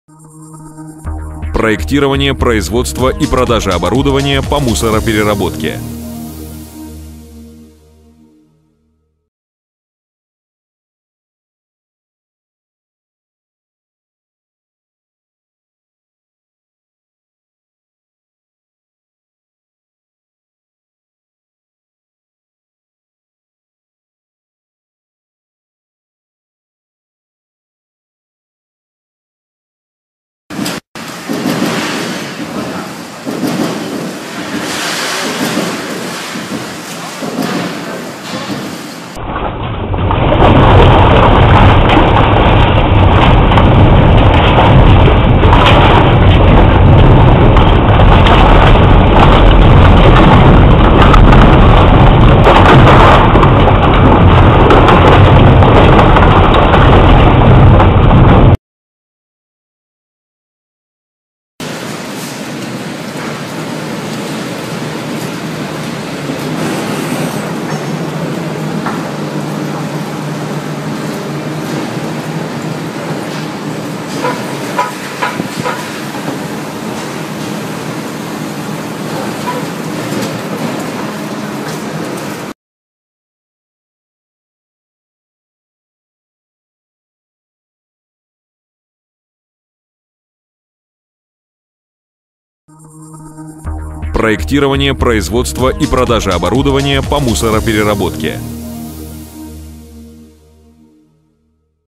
Завод по производству топлива RDF, шредер TRITONE.
Показан завод по производству RDF. Происходит отделение металлов. В решении завода используется шредер TRITONE. Мы рады проконсультировать Вас по необходимому оборудованию и ответить на любые Ваши вопросы. Наш сайт в сети интернет